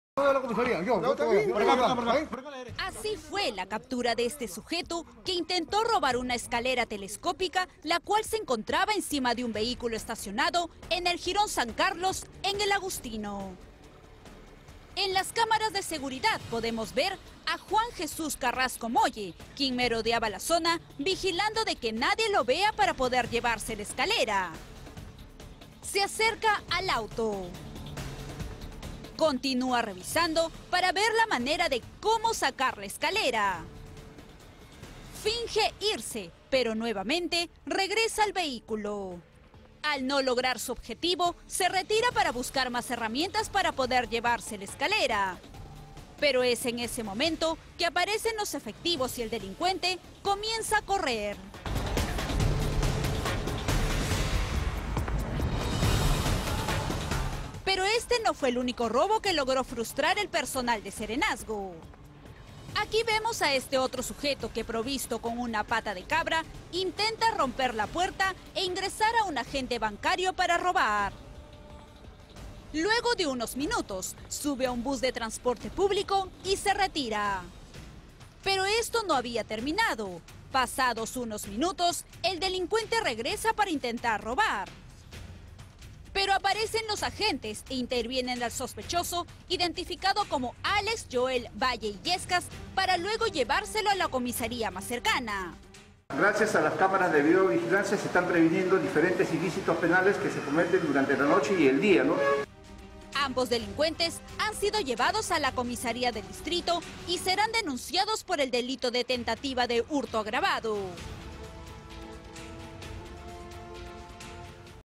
Cae ladrón que intentó robar escalera telescópica en El Agustino
Serenos de El Agustino capturaron a un ladrón que intentó robarse una escalera telescópica que estaba encima de un vehículo estacionado, en el jirón San Carlos.

El delincuente fue identificado como Juan Jesús Carrasco Molle, quien fue detenido cuando merodeaba la zona en uno de sus intentos para llevarse el pesado objeto.

Por otro lado, las autoridades ediles capturaron a Alex Rosibel Joe Valle, un sujeto que fue registrado en imágenes tratando abrir la puerta de un agente bancario para robar.